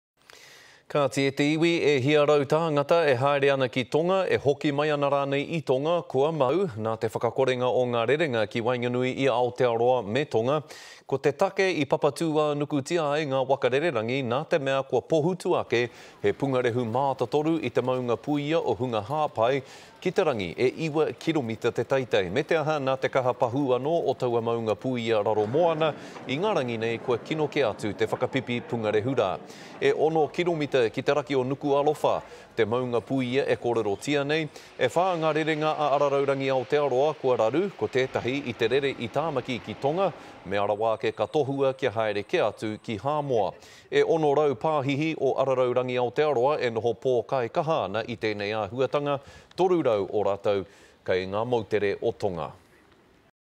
Ash clouds from Tongan volcanoes stranding travellers
Hundreds booked to fly to or return from Tonga are stranded after flights between NZ and the island nation were grounded.
Flights were grounded due to plumes of ash shooting skyward from volcano Hunga Ha'apai, reaching heights of over 2000ft, but with another explosion from the underwater volcano today, the ash cloud is only getting worse.